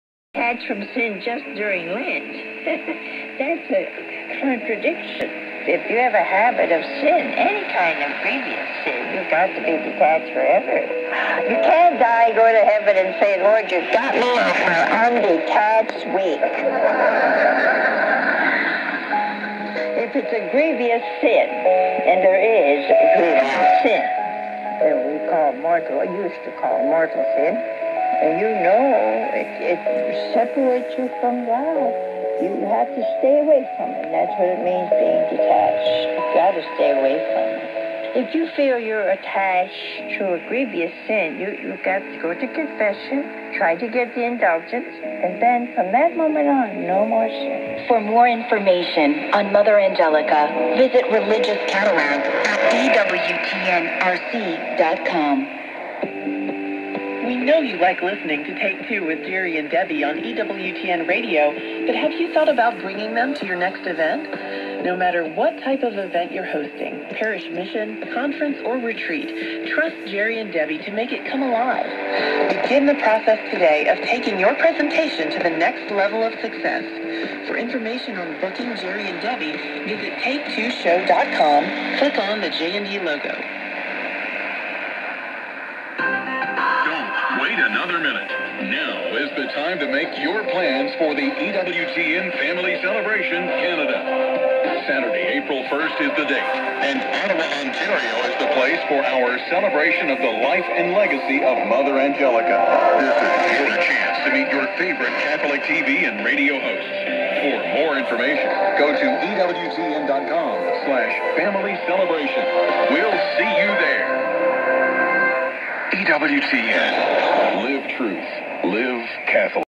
15610 kHz - EWTN (WEWN) (English)
Eternal Word Television Network (WEWN) 15610 kHz - English, - Vandiver,USA
Monday 13 March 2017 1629 UTC
QTH: Bilbao, Bizkaia, SPAIN.
Tecsun PL-880 + DEGEN DE31MS Loop Antenna